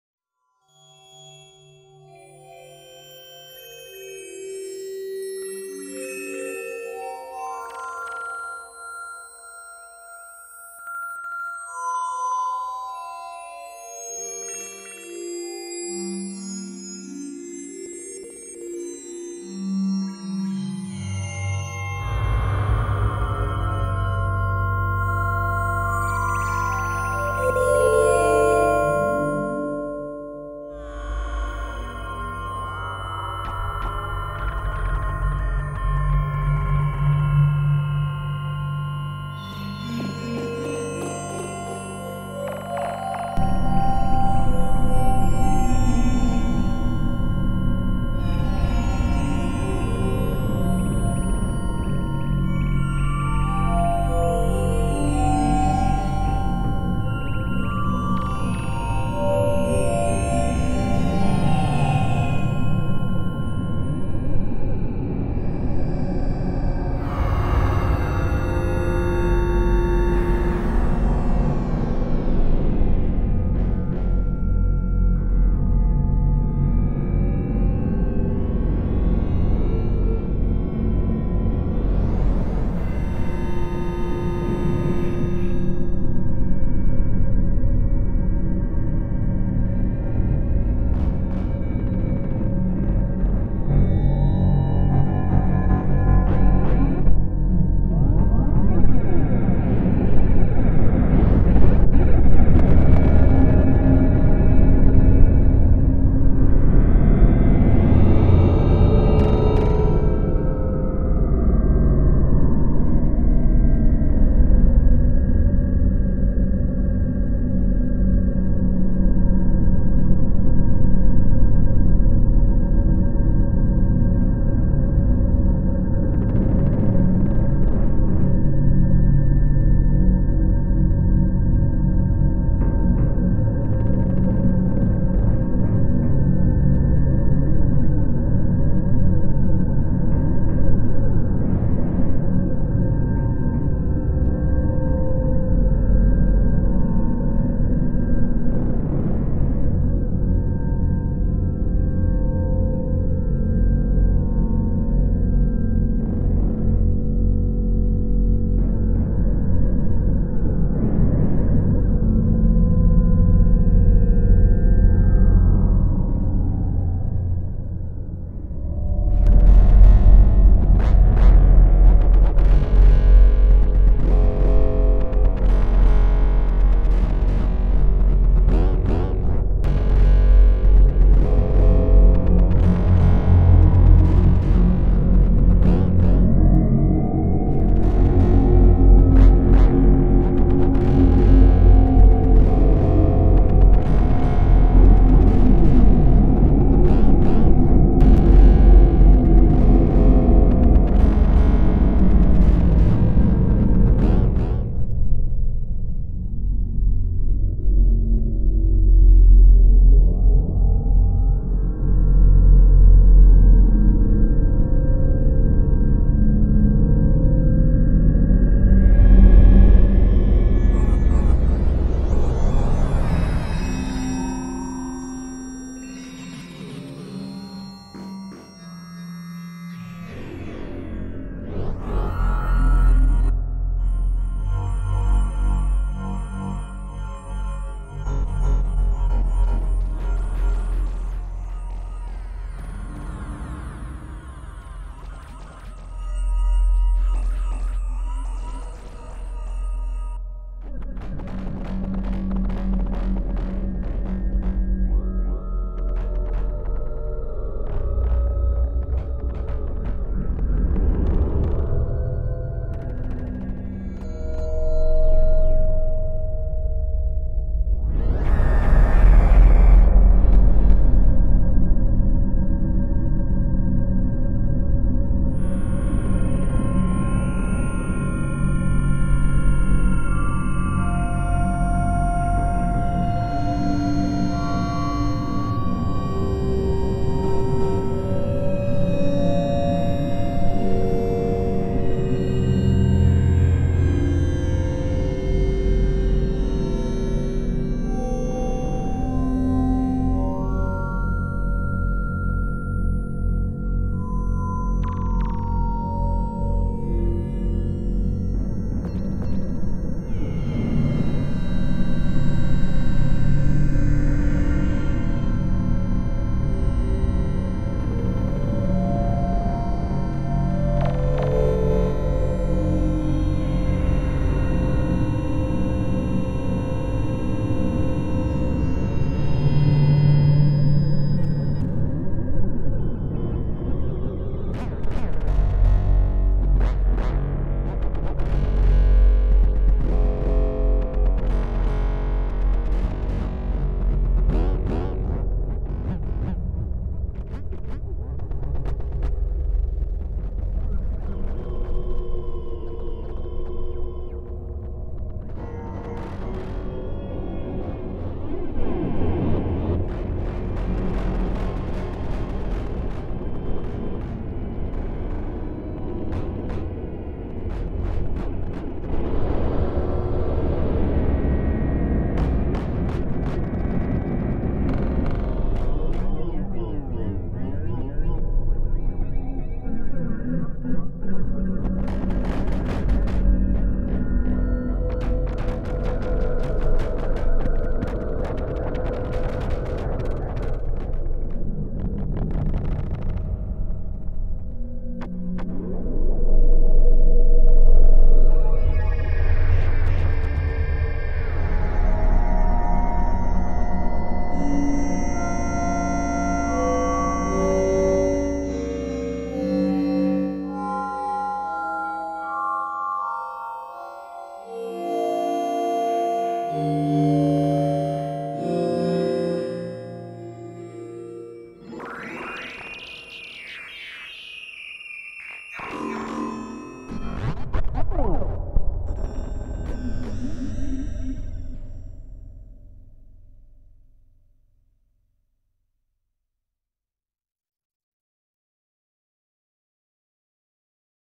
EX5 patch Forest Dadra
A patch i made on my Yamaha EX5 where i use the FDSP engine.